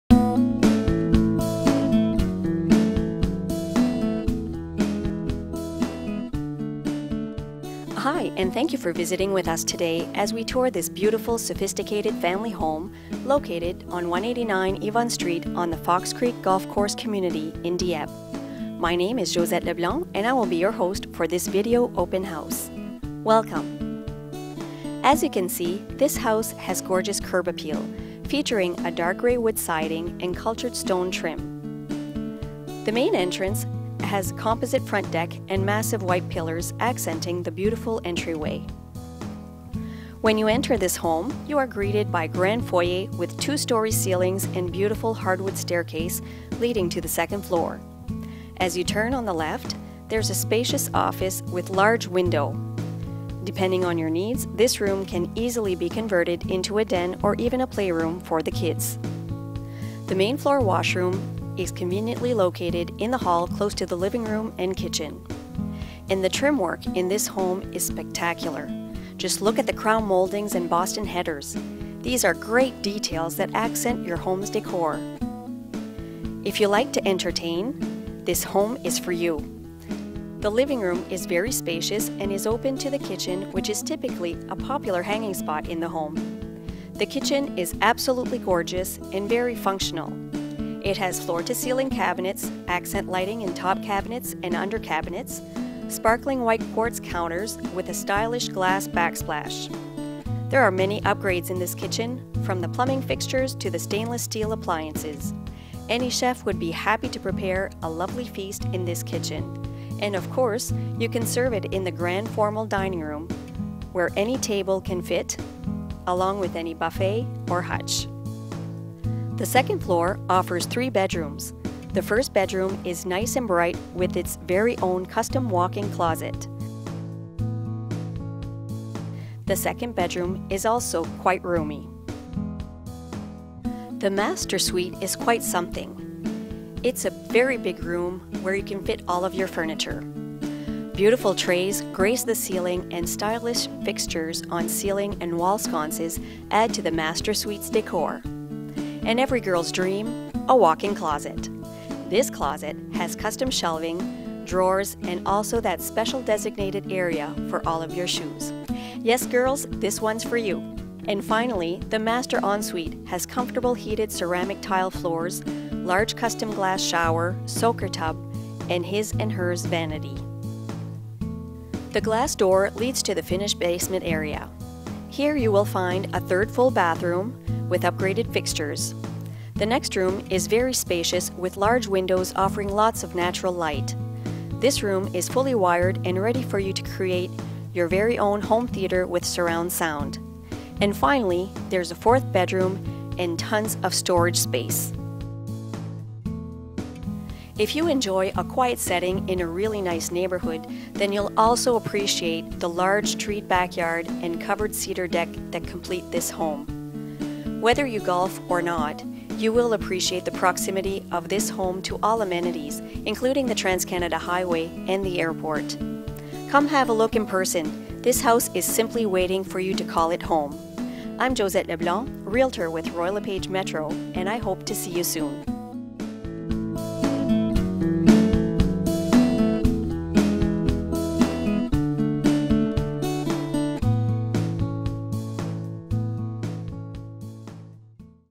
189 rue YVONNE, Dieppe (NB Canada)
SOPHISTICATED FAMILY HOME IN FOX CREEK GOLF COMMUNITY! Approx. 3500sq ft. of living space, this home sits on a large lot with future tee off behind treed backyard. It is energy efficient ($205 avg/mth). The main floor consists of grand foyer with beautiful staircase leading to 2nd storey. Off the main entrance is the office (den/playroom), ½ bath, open concept living rm & kitchen + formal dining rm. The large custom white kitchen offers ample storage, large island w quartz counters + appliances. The 2nd level offers 2 good size bedrooms (1 with WIC); full bath and exquisite master suite with WIC & 5pc ensuite. The basement offers full bath, large family/theater room and 4th bedroom. The back has a covered cedar deck. Extras: quality built (plywood), add'l insulation, heat pump (A/C), central vac; dbl car garage; fireplace, coffered ceilings, 9ft ceiling (main level); heated ensuite floor, composite front deck.